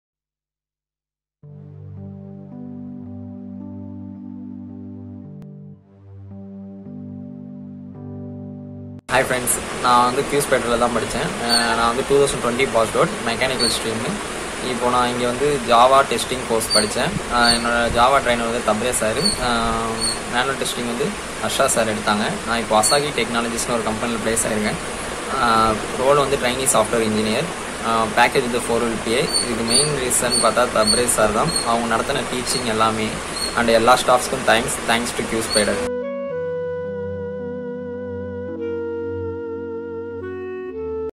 Success story of QSpiders JSpiders Student Venkataguru B | Placed as Software Engineer…
Congratulations to Venkataguru B…
Student of QSpiders (vadapalani) got placed . "Do use the opportunity for your wonderful career start." Attend classes conducted from QSpiders (vadapalani) and got placed . . .